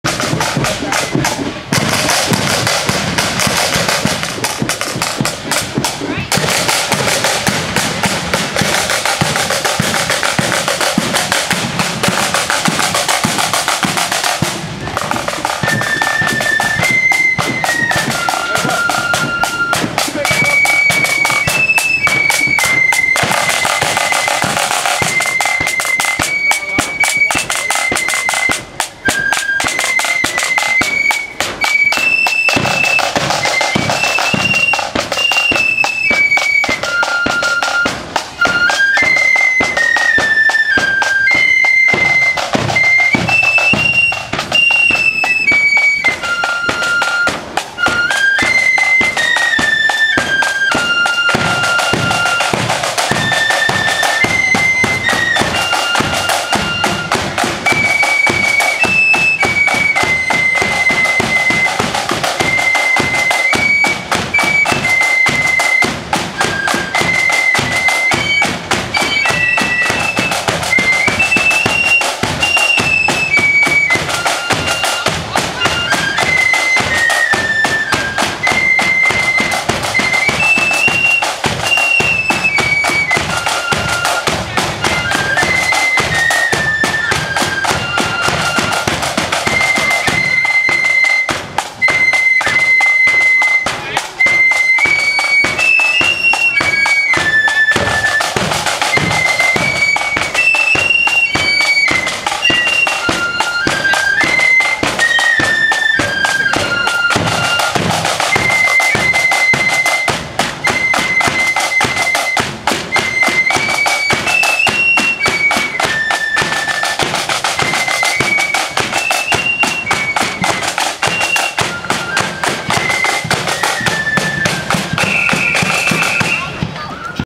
Portadown True Blues FB @ Upper Falls Protestant Boys FB Parade 2016
Portadown True Blues Flute Band on parade at Upper Falls Protestant Boys FB Parade 2016